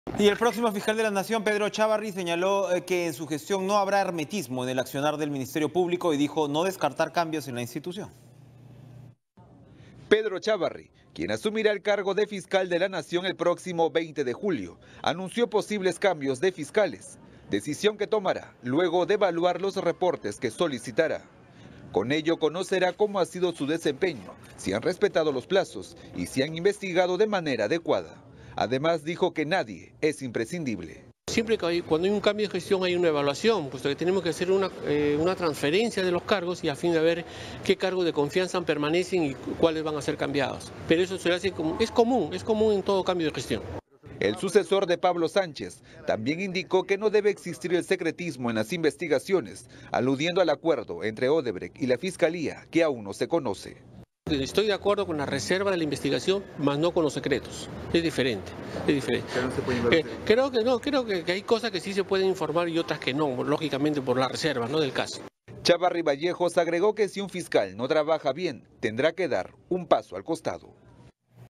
Próximo fiscal de la Nación anuncia evaluación y cambios en el Ministerio Público
El próximo fiscal de la Nación, Pedro Chavarry, anunció posibles cambios de fiscales luego de evaluar los reportes que solicitará.